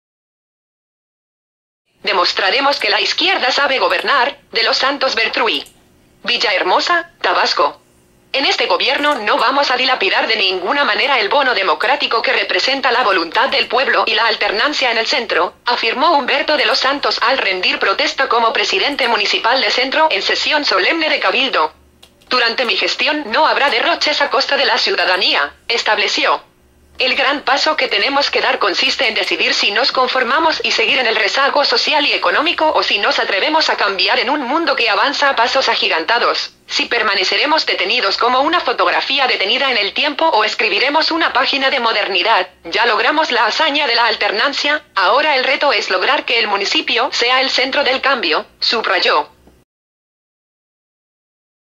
Demostraremos que la izquierda sabe gobernar, De los Santos Bertruy.mpg
Demostraremos que la izquierda sabe gobernar, De los Santos Bertruy.
Villahermosa, Tabasco.- En este gobierno no vamos a dilapidar de ninguna manera el bono democrático que representa la voluntad del pueblo y la alternancia en el Centro, afirmó Humberto de los Santos al rendir protesta como presidente municipal de Centro en Sesión Solemne de Cabildo. "Durante mi gestión no habrá derroches a costa de la ciudadanía", estableció.

"El gran paso que tenemos que dar consiste en decidir si nos conformamos y seguir en el rezago social y económico o si nos atrevemos a cambiar en un mundo que avanza a pasos agigantados, si permaneceremos detenidos como una fotografía detenida en el tiempo o escribiremos una página de modernidad, ya logramos la hazaña de la alternancia, ahora el reto es lograr que el municipio sea el centro del cambio", subrayó.